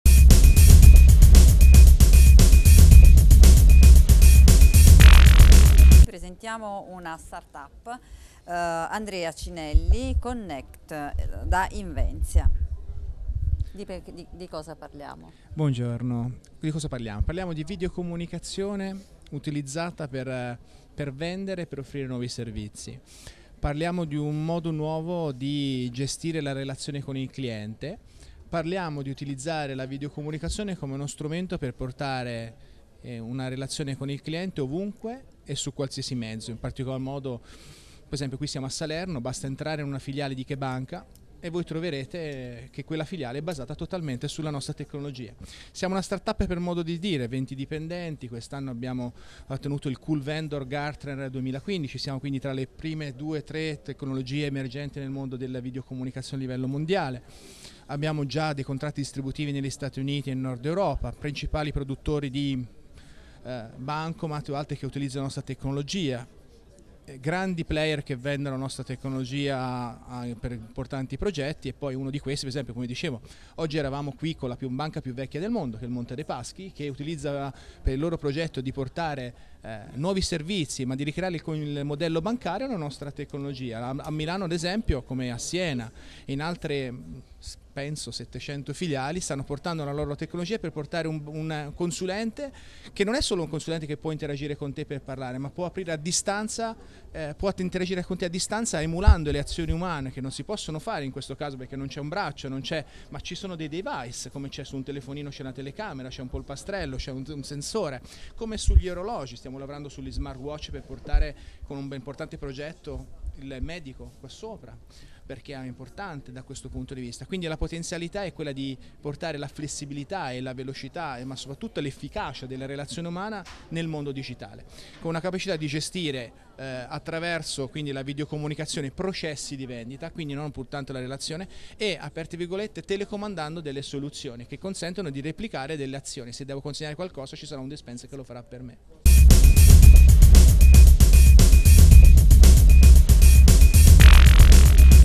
Inventia Srl - Premio Best Practices 2015
Andrea Cinelli presenta Connect - Piattaforma software di video customer engagement